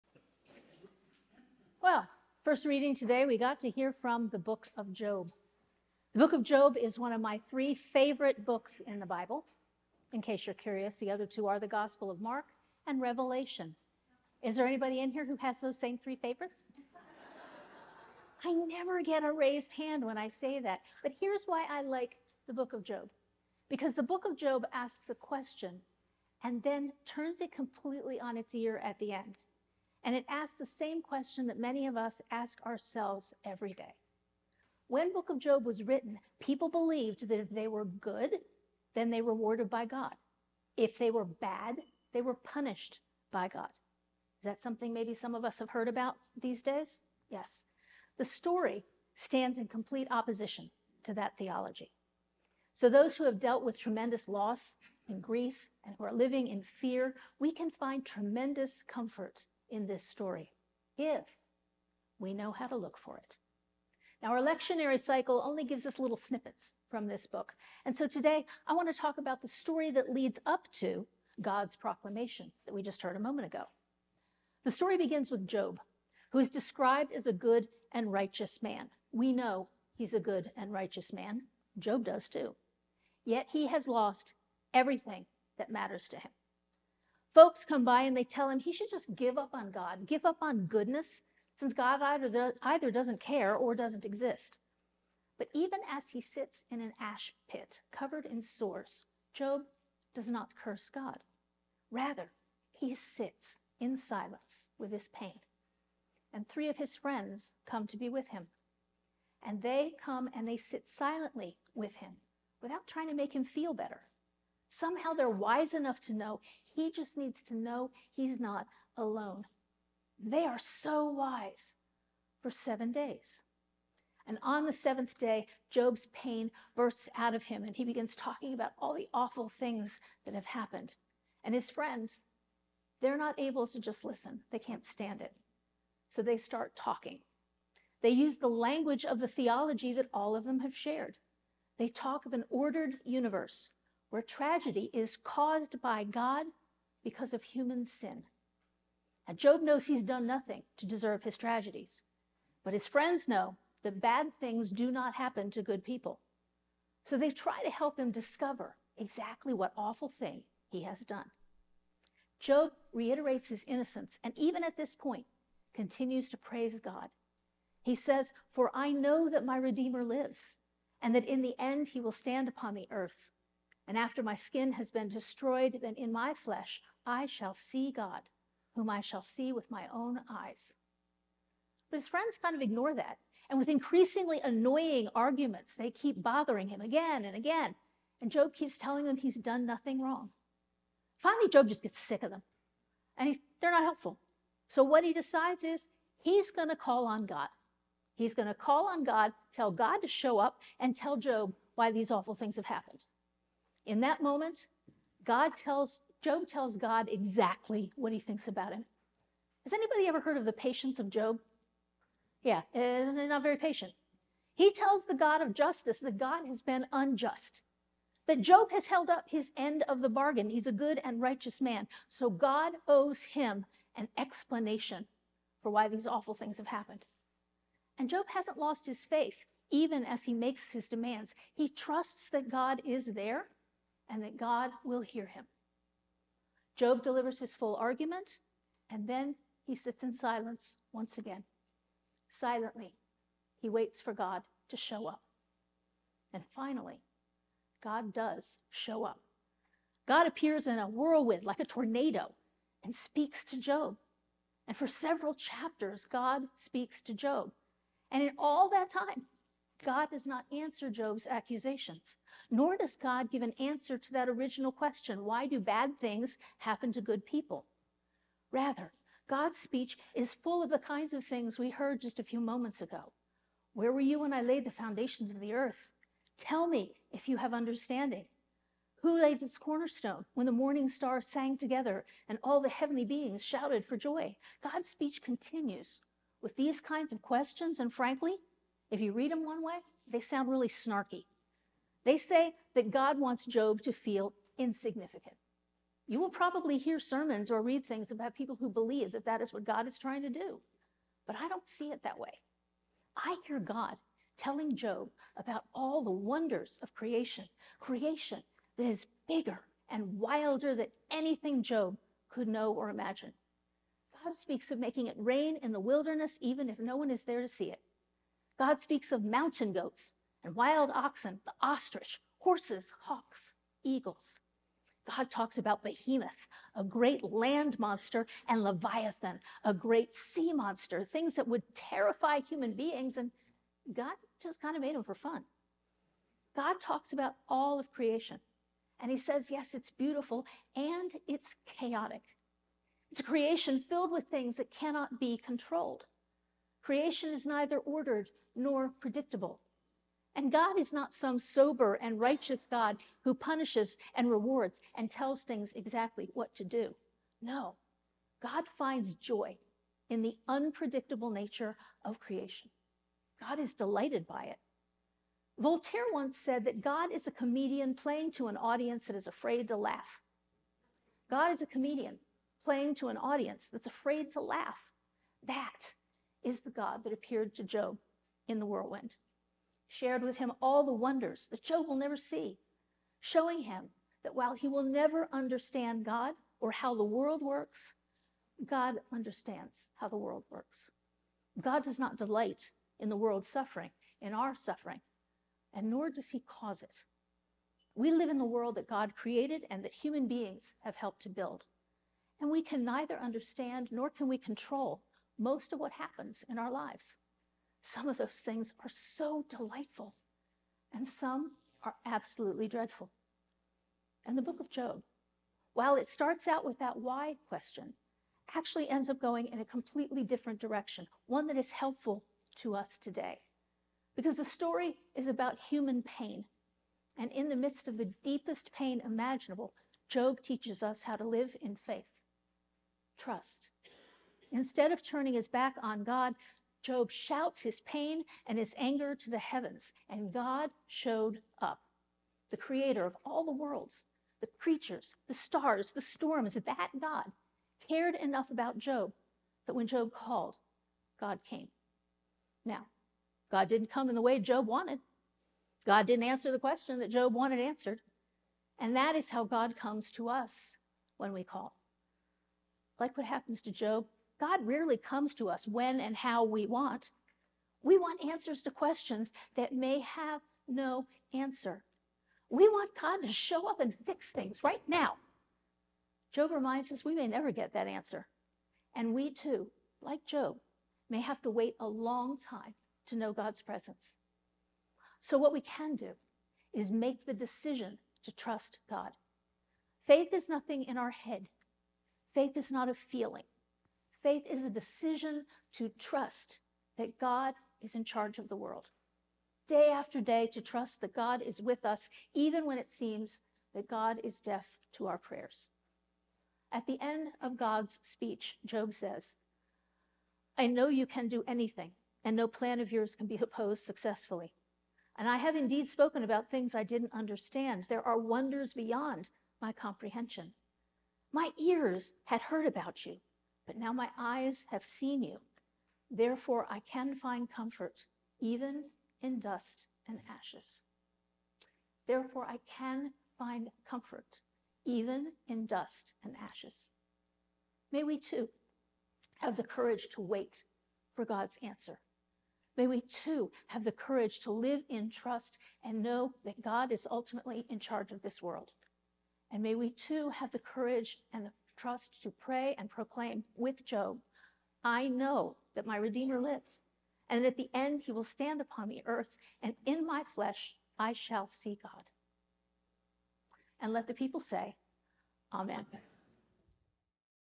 Emmanuel Sermons 22nd Sunday after Pentecost 10 20 2024
Emmanuel Sermons 22nd Sunday after Pentecost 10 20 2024,,The Rev. Joan M. Testin